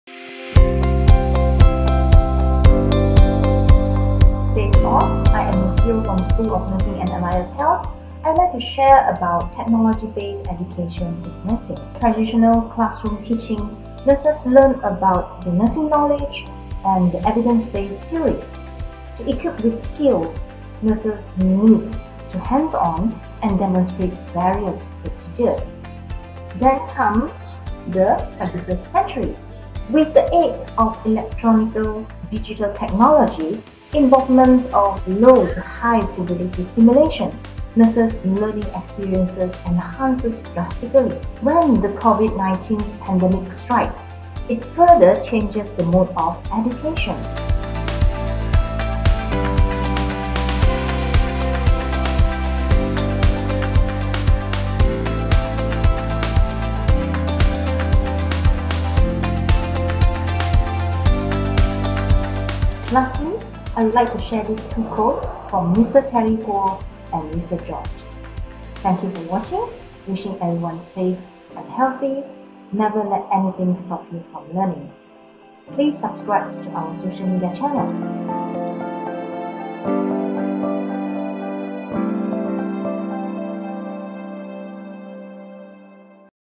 60 seconds to your Future - Technology-Based Education in Nursing
How the traditional classroom teaching change to a technology-based education for Nursing?

Learn what you need to know about technology-based education in Nursing by watching this 60 seconds.